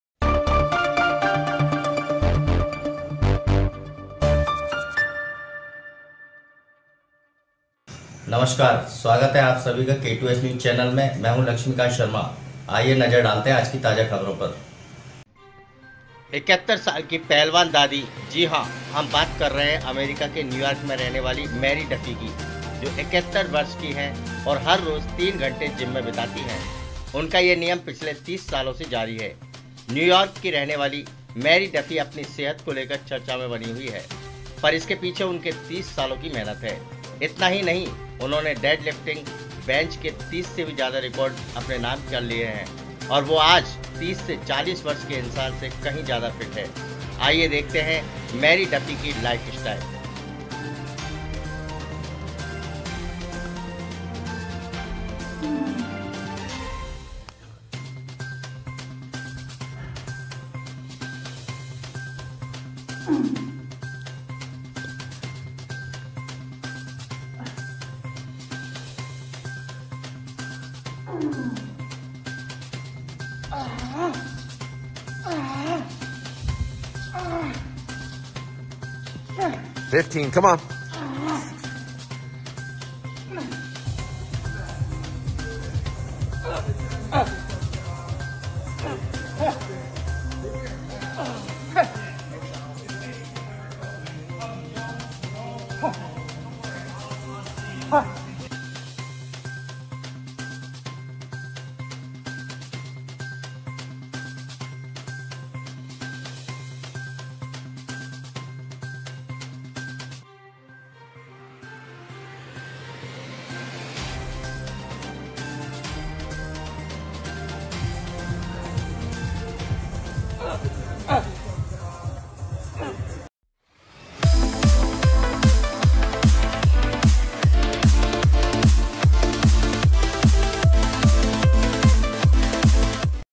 71 साल की पहलवान दादी जो सबको धूल चटा दे ।
71 साल की पहलवान दादी जो सबको धूल चटा दे ।